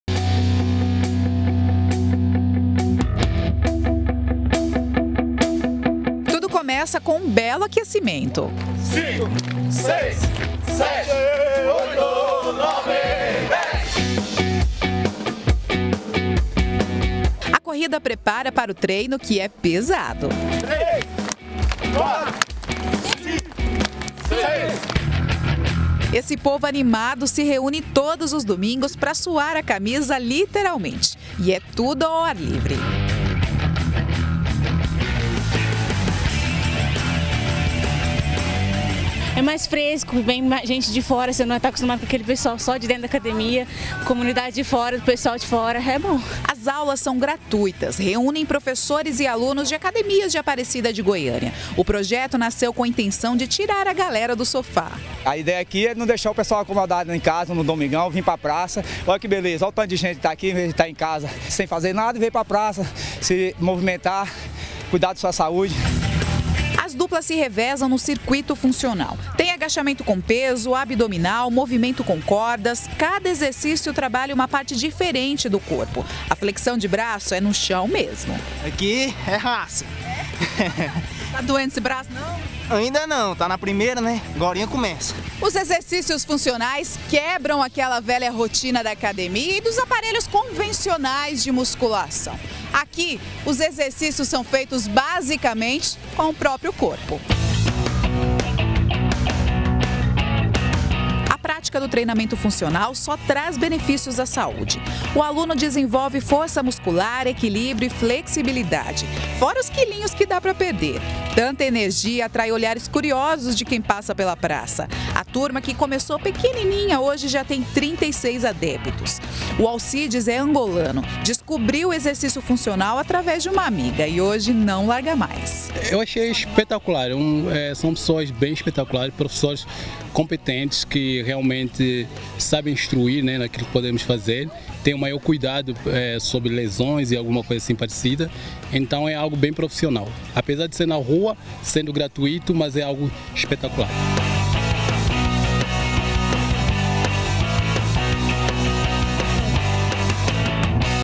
MORADORES SE REÚNEM PARA PRATICAR EXERCÍCIOS
Moradores do Village Garavelo, em Aparecida de Goiânia, se reúnem todo domingo para sair do sedentarismo. Eles fazem atividades funcionais num parque da região. Tudo muito organizado e com orientação de professores.